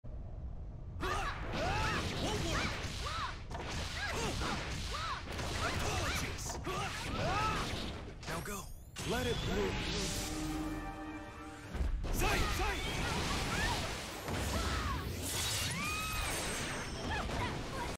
Anji Bombo (awesome)
(anti air)5H 236[S], c.S (minor delay, for spacing)236S, j.D (delay, for meaty)7j.D, 2S 5H 236P, 63214H